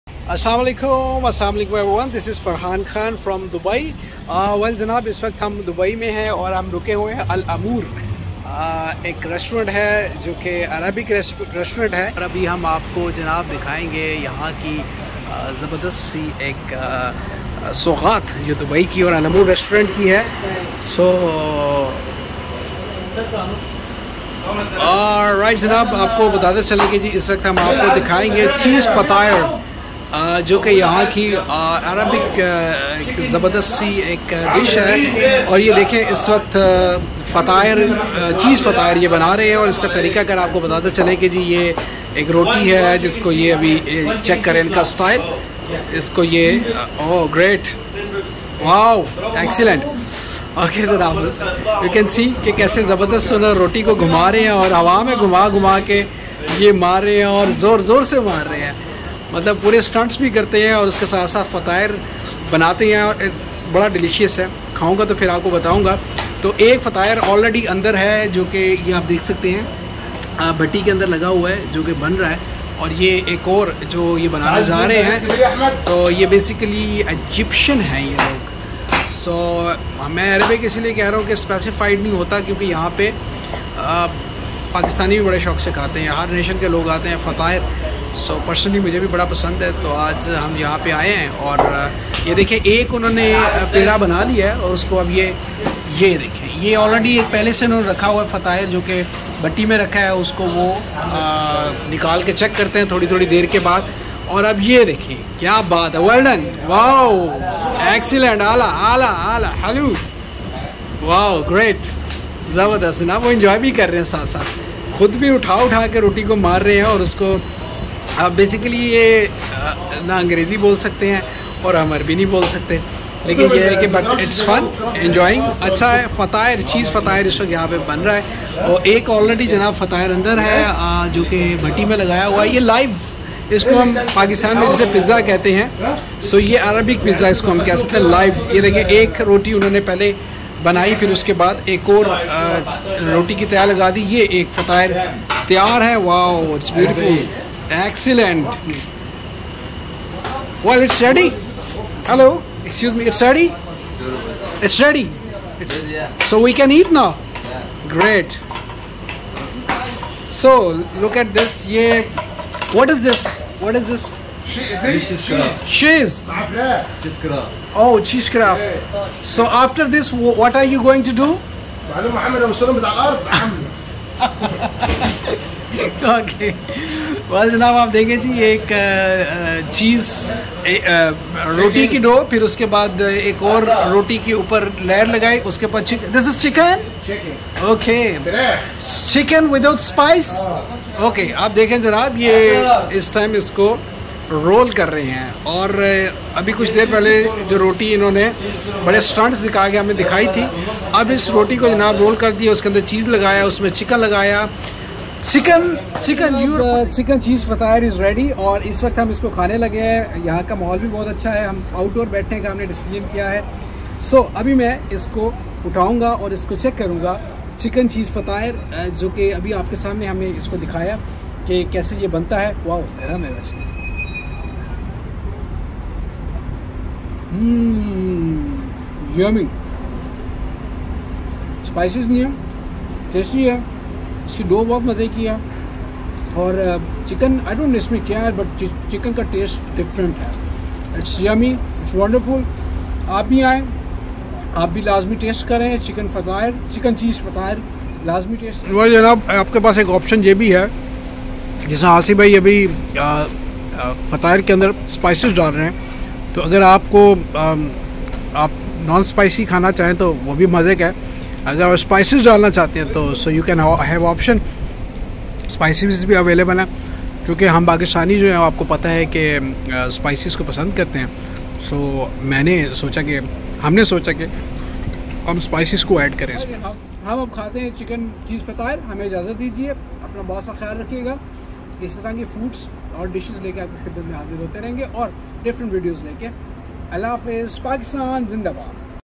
How to make Chicken Cheese Fatair In Dubai
The area is famous for its rich tastes and expensive lifestyle, and you'll spend a small fortune if you eat in nice restaurants every single night. Street food is an inexpensive and tasty option, and many people who live in the area eat street food every day.